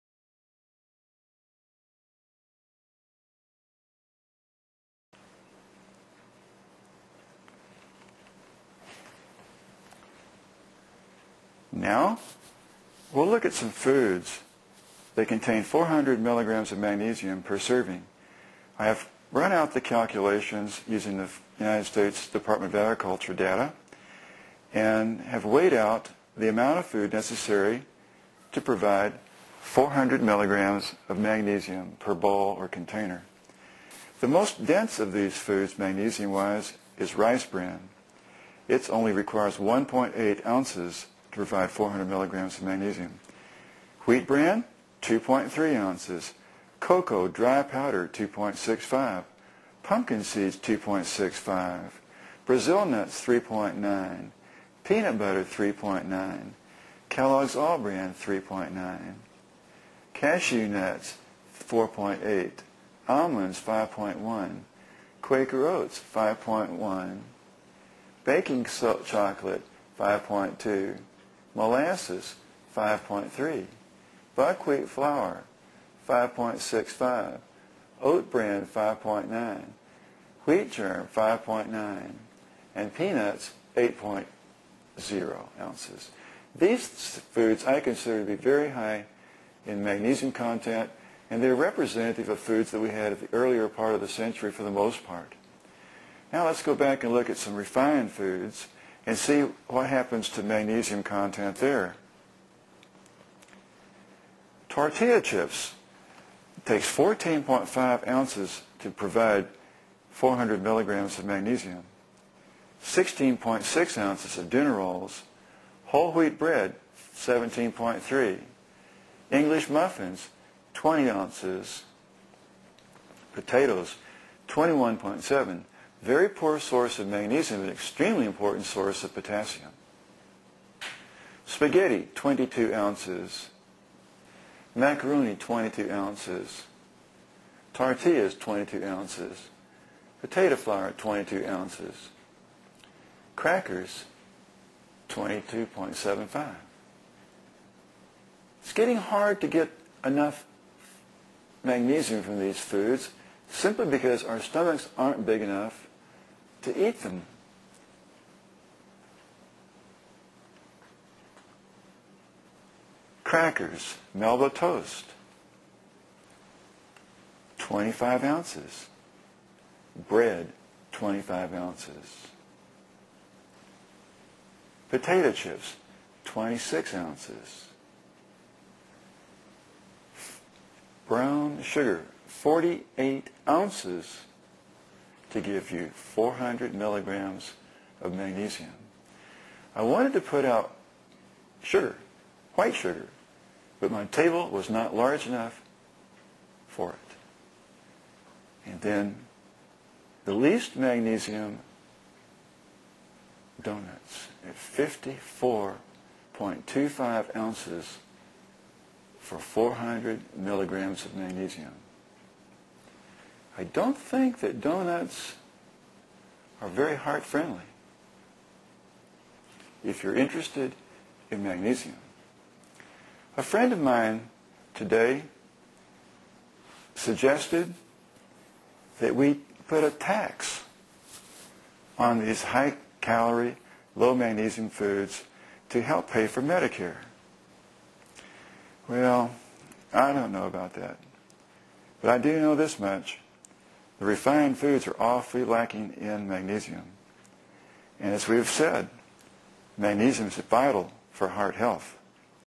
foods containing 400mg magnesium
The amount of a number of foods containing the RDA of 400 mg of magnesium are shown.